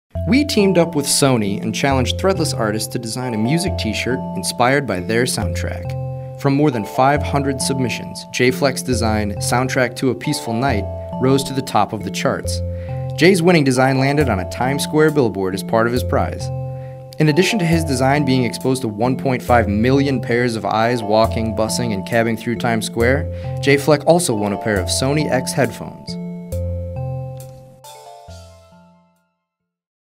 Jay Fleck's "Soundtrack to a Peaceful Night" up on the Sony billboard in Times Square!
Congrats to Jay Fleck! His design Soundtrack to a Peaceful Night was chosen as the Sony Soundtrack winner! Along with $5,000 cash, Sony X Headphones, and concert tickets, he also won the opportunity to have his design displayed right smack in the center of Times Square for a week this past December. For more info on the challenge head over